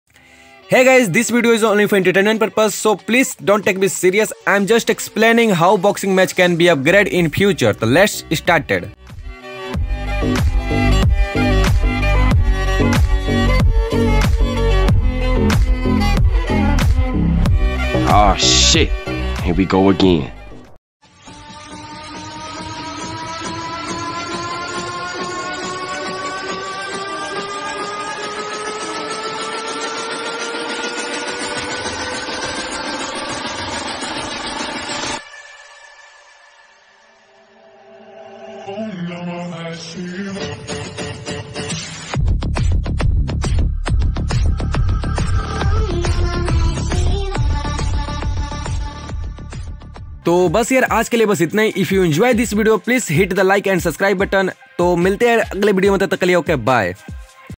Past boxing vs Present boxing vs Future boxing match... Just imagination.
I am just explaining how boxing match can upgrade in future. its my imagination.
I uploaded   ||   funny videos, imagination video, etc.